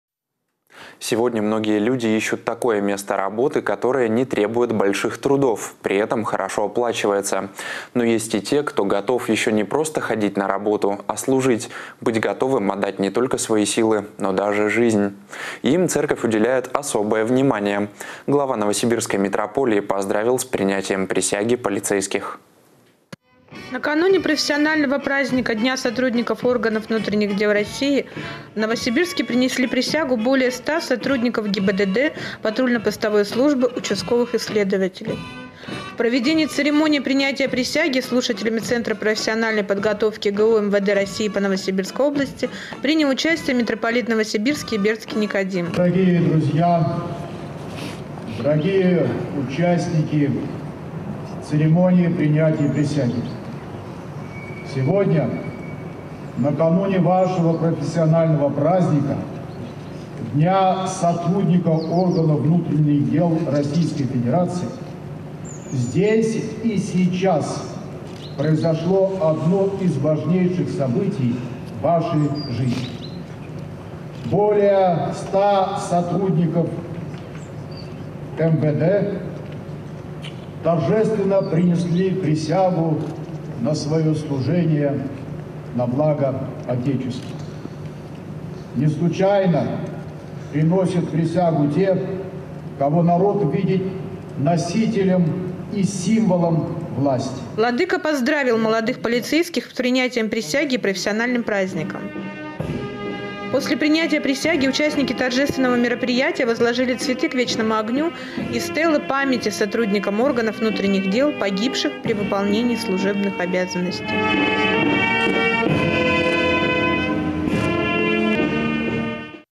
Глава Новосибирской митрополии поздравил с принятием присяги полицейских.
Сегодня многие люди ищут такое место работы, которое не требует больших трудов, при этом хорошо оплачивается. Но и есть и те, кто готов еще не просто ходить на работу, а служить, быть готовым отдать не только свои силы, но даже жизнь. И им церковь уделяет особое внимание. Глава Новосибирской митрополии поздравил с принятием присяги полицейских.